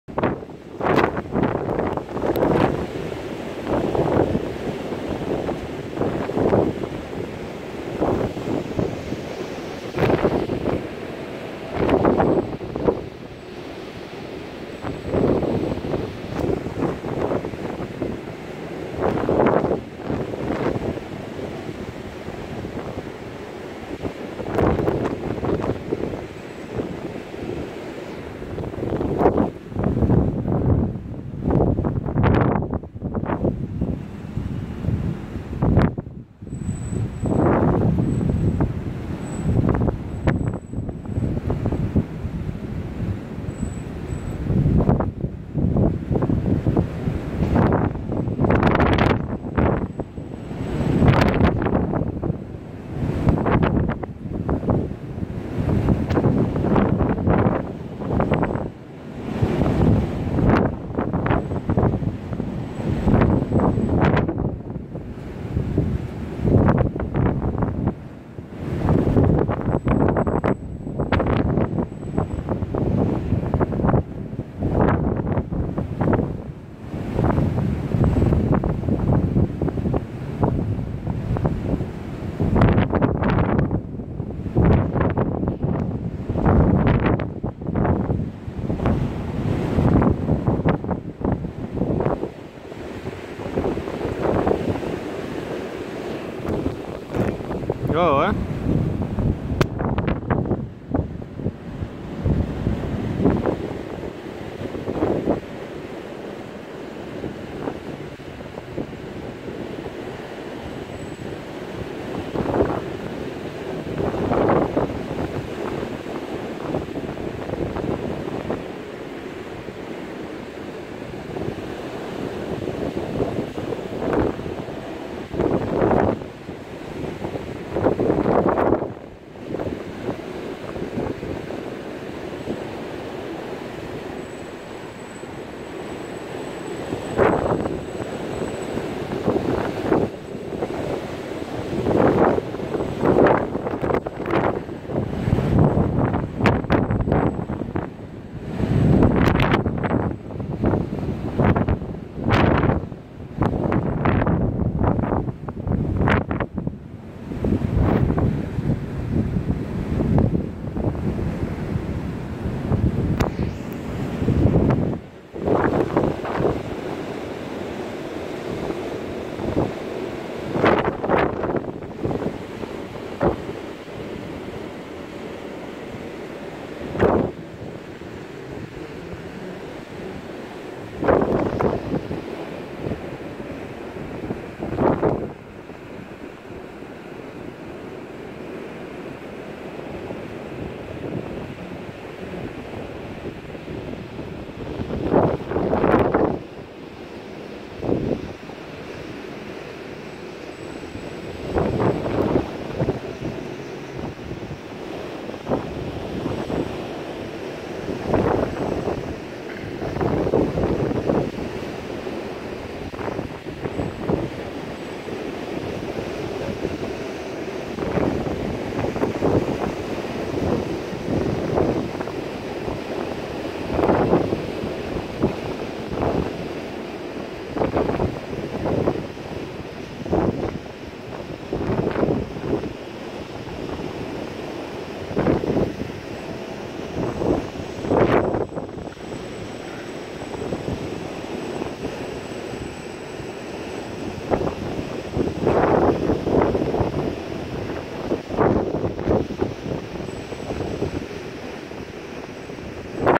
Vol pente forca real (66) avec planeur kinetic 800
Petit vol de pente à força real dans le 66 pour moi une première en pente pur avec du vent très fort!!!